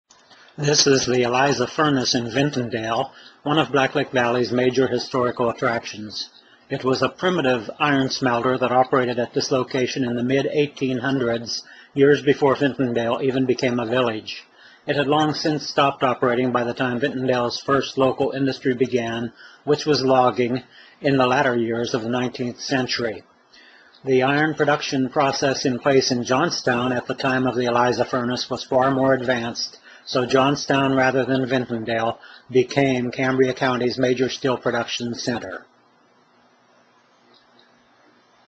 Eliza Furnace, Vintondale PA
Historic iron smelting furnace. submitted by Jon Kennedy.